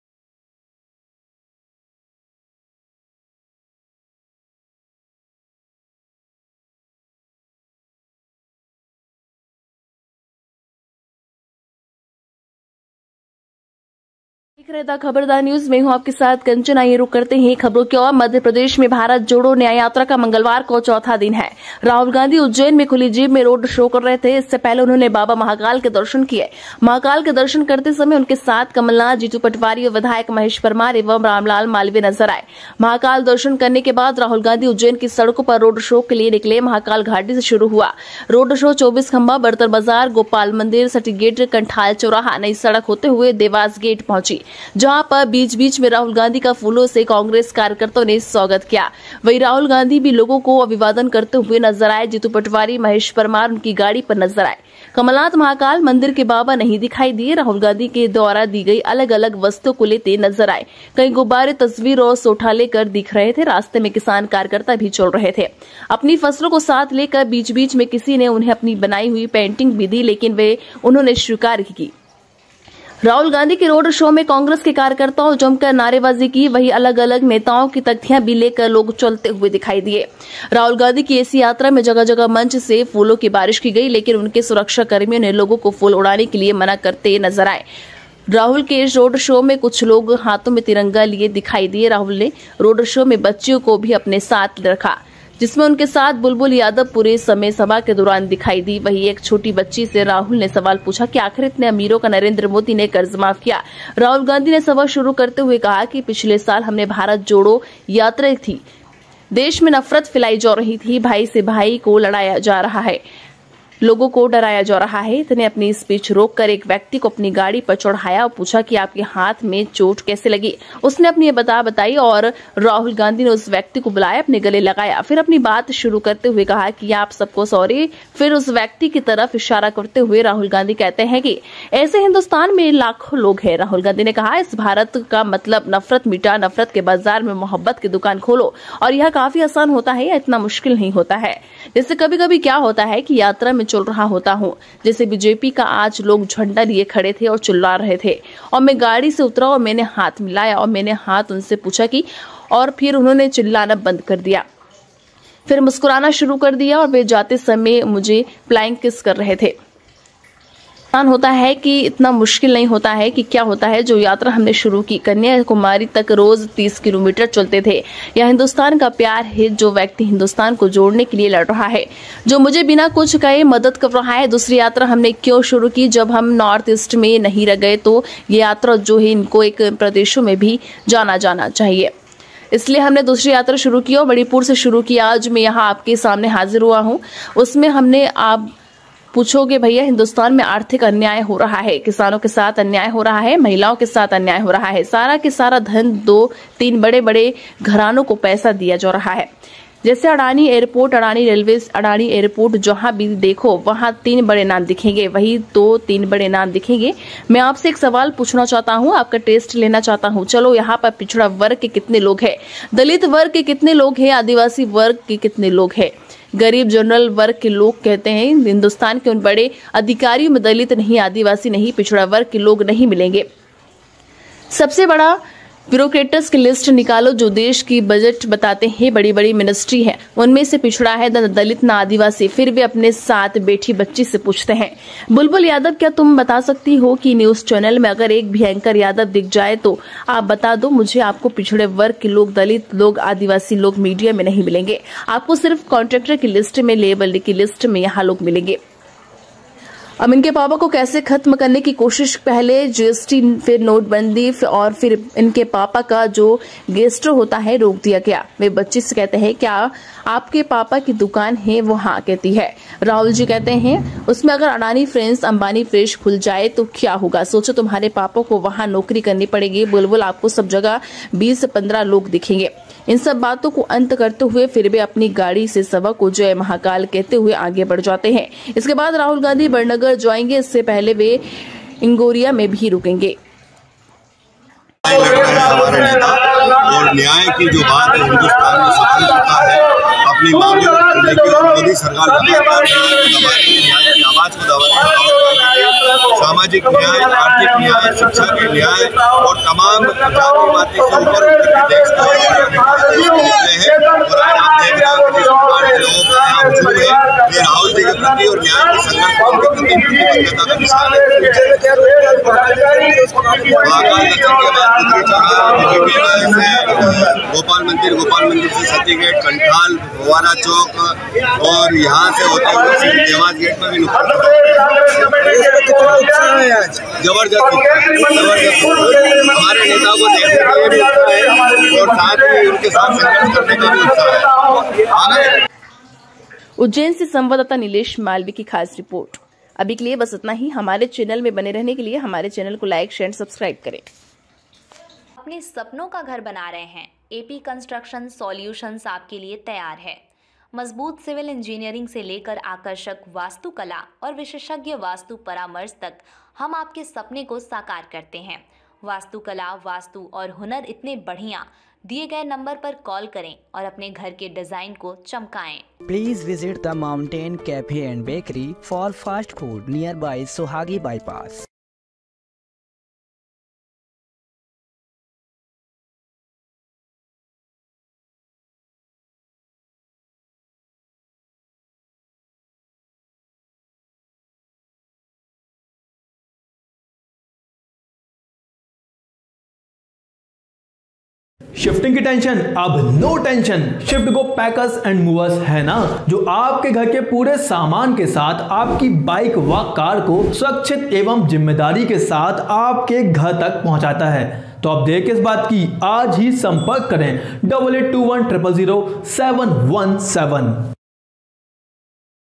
मध्यप्रदेश में भारत जोड़ो न्याय यात्रा: राहुल गांधी का चौथा दिन,उज्जैन में रोड शो के साथ महाकालदर्शन
राहुल गांधी का चौथा दिन: उज्जैन रोड शो में महाकाल दर्शन से लेकर अद्वितीय भारत यात्रा
मंगलवार (मंगलवार) को मध्य प्रदेश में भारत जोड़ो न्याय यात्रा का चौथा दिन है। राहुल गांधी उज्जैन में खुली जीप में रोड शो कर रहे थे. इससे पहले उन्होंने महाकाल मंदिर में दर्शन किए, जहां उनके साथ कमल नाथ, जीतू पटवारी, विधायक महेश परमार और राम लाल मालवीय भी थे। महाकाल में आशीर्वाद लेने के बाद, राहुल गांधी ने उज्जैन में महाकाल घाटी से रोड शो शुरू किया, जो चौबीस खंबा, बरटन बाजार, गोपाल मंदिर, सती गेट, कंठाल चौराहा जैसे विभिन्न स्थलों से गुजरते हुए देवास गेट पहुंचे।

रोड शो के दौरान कांग्रेस समर्थकों ने राहुल गांधी का फूलों से स्वागत किया और उन्होंने भी अभिनंदन का जवाब दिया. राहुल गांधी को लोगों के साथ बातचीत करते हुए, गुब्बारे, तस्वीरें और मालाएं जैसी विभिन्न वस्तुएं प्राप्त करते हुए देखा गया। किसान भी अपनी उपज लेकर साथ चल रहे थे। एक उदाहरण में, एक बच्चे ने राहुल गांधी से पूछा कि प्रधान मंत्री नरेंद्र मोदी ने अमीरों के लिए ऋण माफ क्यों किया, लेकिन गरीबों के लिए नहीं। राहुल गांधी ने माफी मांगी और भारत में लोगों को जोड़ने का महत्व समझाया.

राहुल गांधी ने नफरत को खत्म करने और प्यार का बाजार बनाने की जरूरत पर जोर दिया. उन्होंने भारत जोड़ो यात्रा के दौरान अपने अनुभवों को याद किया और ऐसे उदाहरणों पर प्रकाश डाला जहां विभिन्न पृष्ठभूमि के लोगों ने हाथ मिलाया। उन्होंने अपनी यात्रा की घटनाओं को साझा किया, जिसमें भाजपा समर्थकों के साथ मुठभेड़ भी शामिल थी, और देश में एकता को बढ़ावा देने के महत्व पर जोर दिया।

पूरी यात्रा के दौरान मंच से फूल बरसाने की घटनाएं हुईं, हालांकि सुरक्षाकर्मियों ने लोगों को राहुल पर फूल फेंकने से रोका. चुनौतियों के बावजूद, रोड शो जारी रहा और राहुल गांधी ने लोगों से बातचीत की, उनकी चिंताओं को दूर किया और एकता के संदेश को बढ़ावा दिया।

राहुल गांधी ने आर्थिक अन्याय, किसानों और महिलाओं के खिलाफ अन्याय के मुद्दे उठाकर अपनी बात समाप्त की और बड़े निगमों को धन आवंटन पर सवाल उठाया। उन्होंने लोगों से शीर्ष नौकरशाही पदों और मीडिया में हाशिए पर रहने वाले समुदायों के प्रतिनिधित्व के बारे में सोचने का आग्रह किया।

विवरण राहुल गांधी की बड़ी नगर यात्रा और यात्रा जारी रखने से पहले इंदौर में रुकने की उनकी योजना के साथ समाप्त होता है।''